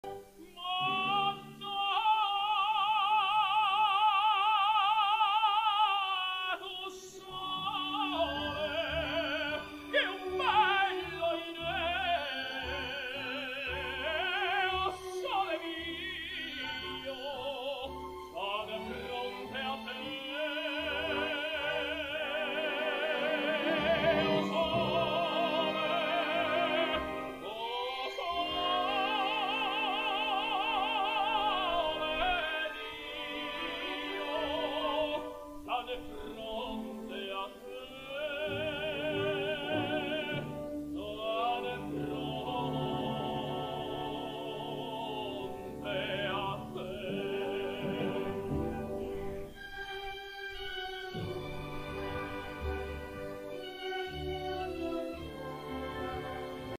Francesco Demuro y Nadine Sierra 3
Material de prensa enviado por el Festival Cap Rocat

Publicado en la nota:
Nadine Sierra y Francesco Demuro ponen el broche de oro a la edición 2023 del Festival Cap Rocat